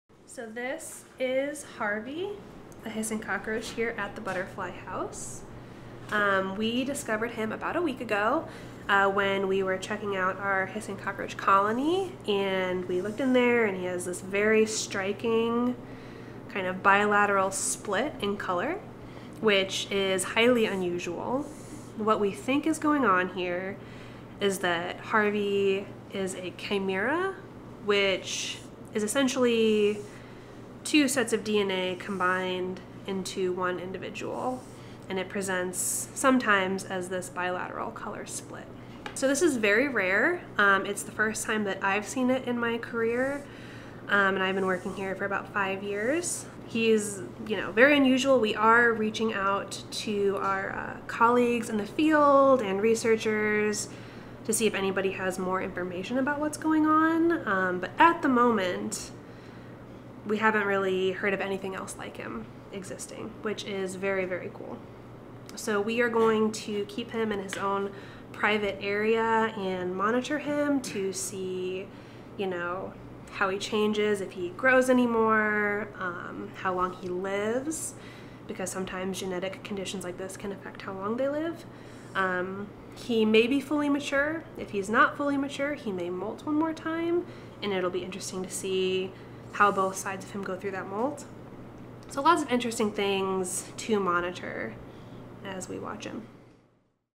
St. Louis scientists find rare mutant cockroach in the Butterfly House's lab
Meet Harvey, the Madagascar hissing cockroach who is split in half, with two distinct colorations. Butterfly House scientists are trying to learn more about the rare mutation.